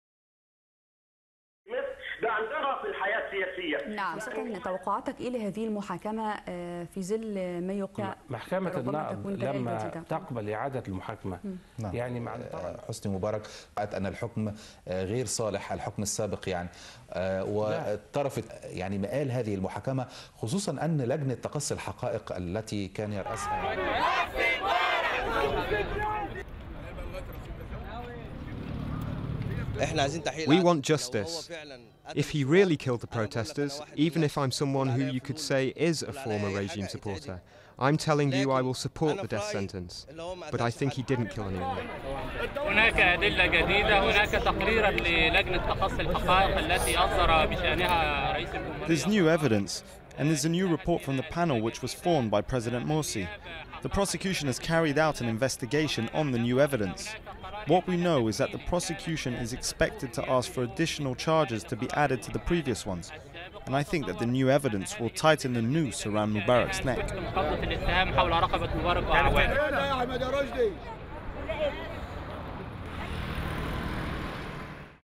Egypt's Mubarak waves and smiles as retrial opens
The trial of Egypt's Hosni Mubarak opened with a chaotic hearing Saturday, lasting just seconds as a combative Mubarak smiled and waved in the dock. Mubarak, his former interior minister Habib al-Adly and six security chiefs were again in the dock for their alleged complicity in the murder and attempted murder of hundreds of peaceful protesters on January 25-31, 2011. Duration: 01:15